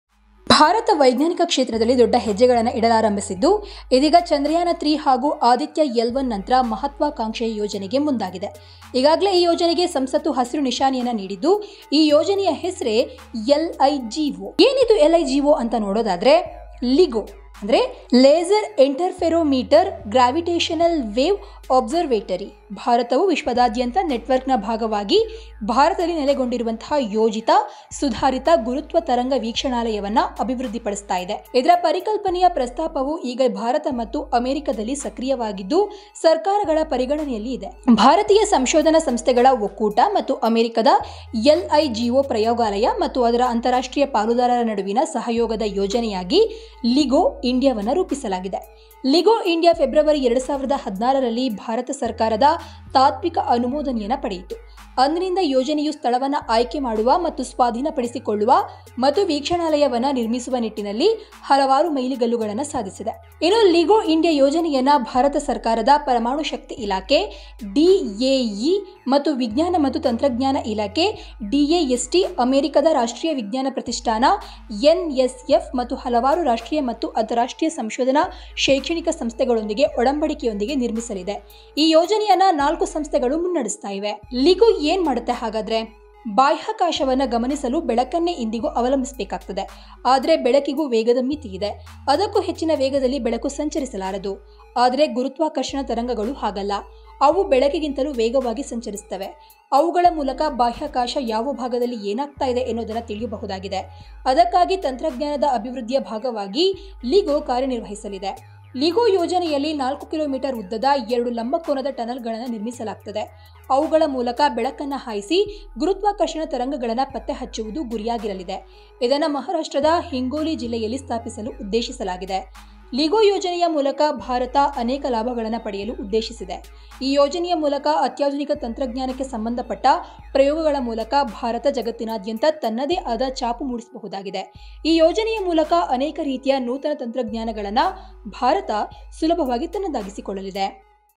ISRO Launched India's First Sun Mission Chandrayaan-3, ಆದಿತ್ಯ ಎಲ್-1 ನಂತರ LIGO
ಭಾರತ ವೈಜ್ಞಾನಿಕ ಕ್ಷೇತ್ರದಲ್ಲಿ ದೊಡ್ಡ ಹೆಜ್ಜೆಗಳನ್ನು ಇಡಲಾರಂಭಿಸಿದ್ದು, ಇದೀಗ ಚಂದ್ರಯಾನ-3 ಹಾಗೂ ಆದಿತ್ಯ ಎಲ್- 1 ನಂತರ ಮಹತ್ವಾಕಾಂಕ್ಷೆಯ ಯೋಜನೆಗೆ ಮುಂದಾಗಿದೆ. ಈಗಾಗಲೇ ಈ ಯೋಜನೆಗೆ ಸಂಸತ್ತು ಹಸಿರು ನಿಶಾನೆಯನ್ನೂ ನೀಡಿದೆ. ಈ ಯೋಜನೆಯ ಹೆಸರೇ LIGO!India has started making big strides in the field of science and is now embarking on an ambitious project after Chandrayaan-3 and Aditya L-1. Parliament has already given green signal to this project. The name of this project is LIGO!